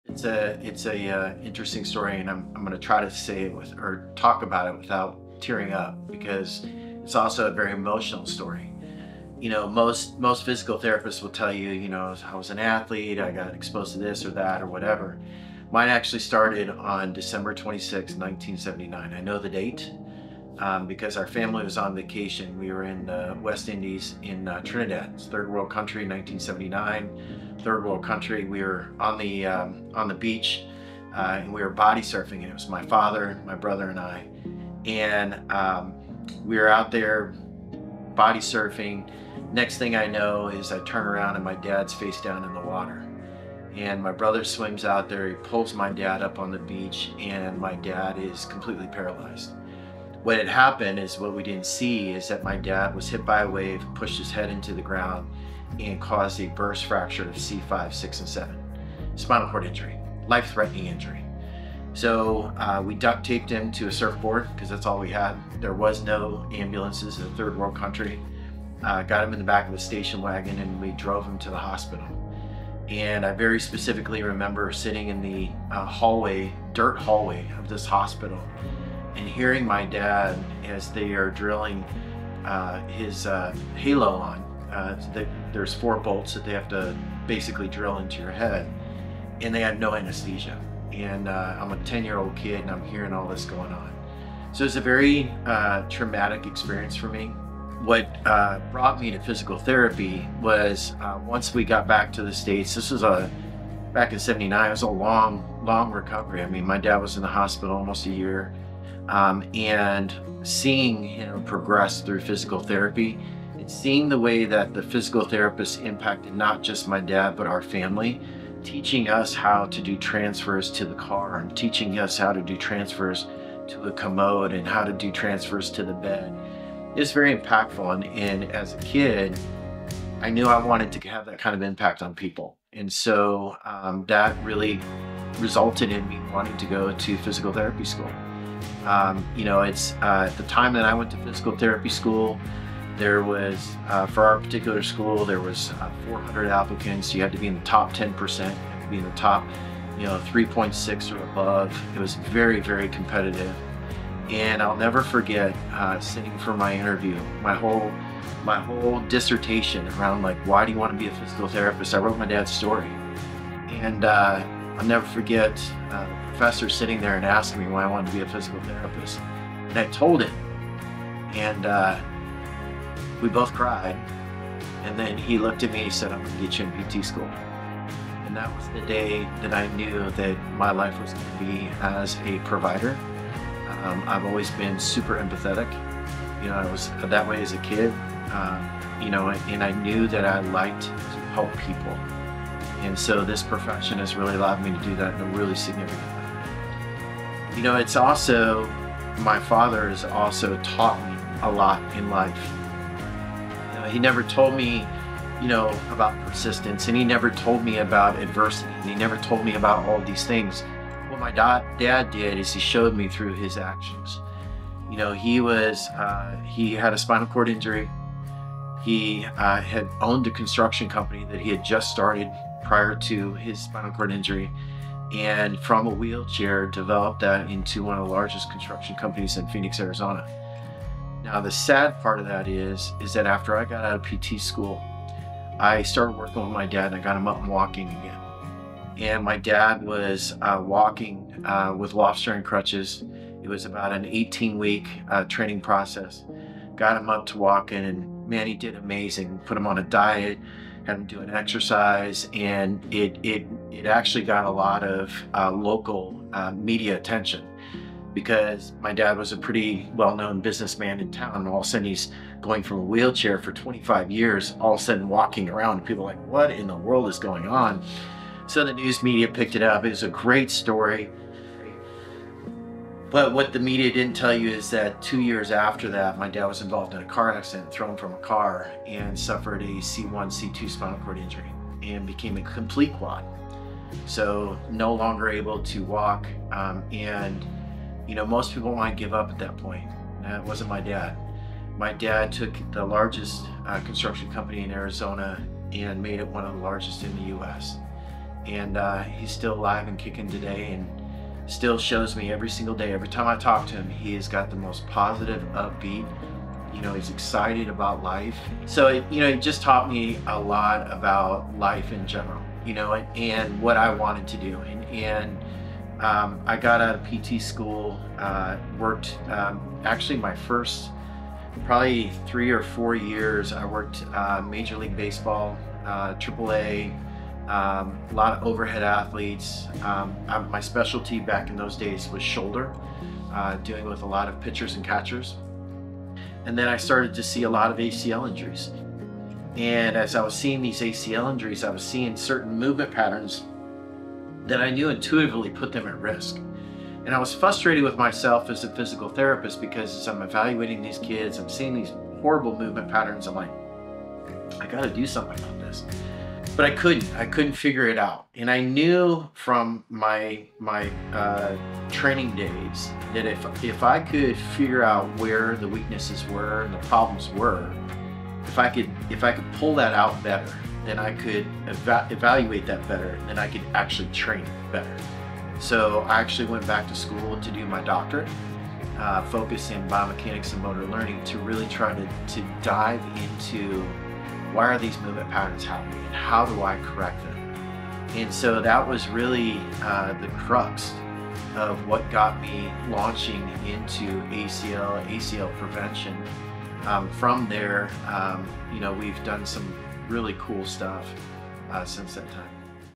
My Why - My Story Behind Why I Do What I Do
Most people know me as a PT. Few people know my story. What is it that drove me to be a PT. What drives me to do what I do. What internal motivation I have to constantly drive and push things forward. This inspirational story started with a devastating accident on Dec. 26, 1979 when my father broke his neck and became a quadriplegic on a family vacation. My father has shown me throughout his life that no barrier is too big. That YOU can accomplish whatever you want as long as you WANT it bad enough. The Athlete Lab is the next chapter in my journey thanks to the example my dad set for me starting in 1979. This one is for you DAD!